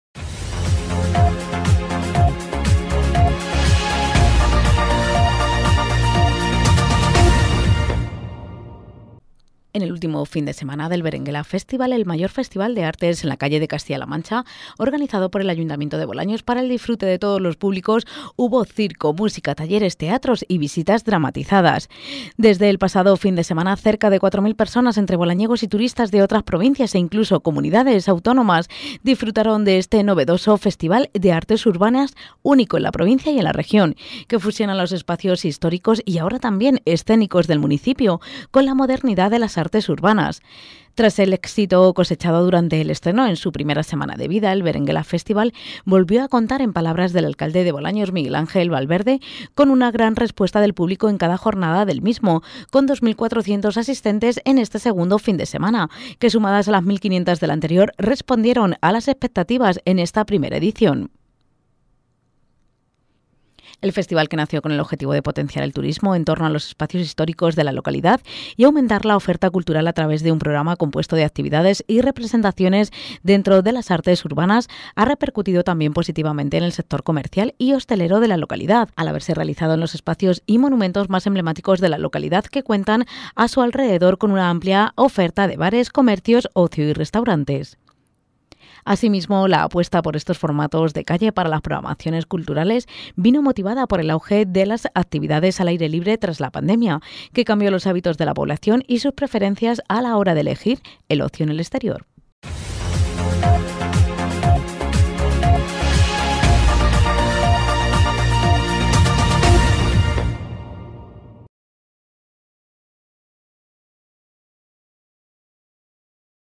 BERENGUELA FESTIVAL 1ª EDICIÓN
Bolaños de Calatrava ha acogido durante los dos últimos fines de semana un festival de artes escénicas en la calle bajo el nombre de Berenguela Festival. Todo un éxito de participación.